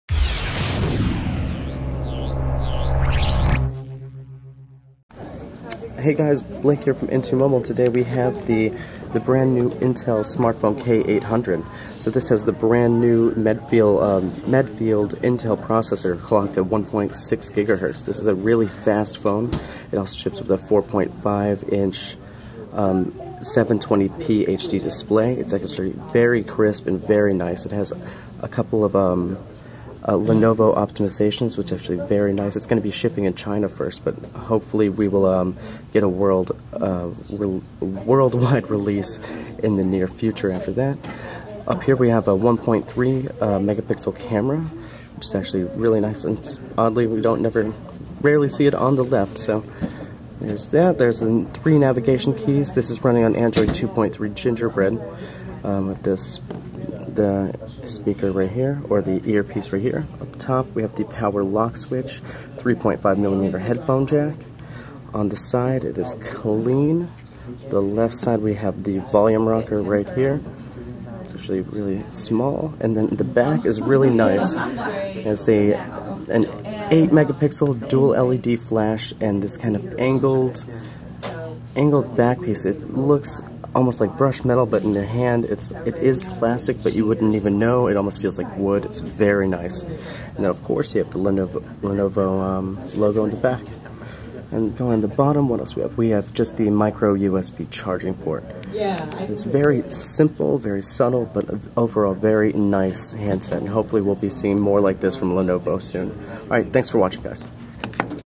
CES 2012: Lenovo K800 with Intel processor
We get a look at what should be the first smartphone with the new Intel Atom chipset for mobile and the Lenovo K800 looks pretty slick.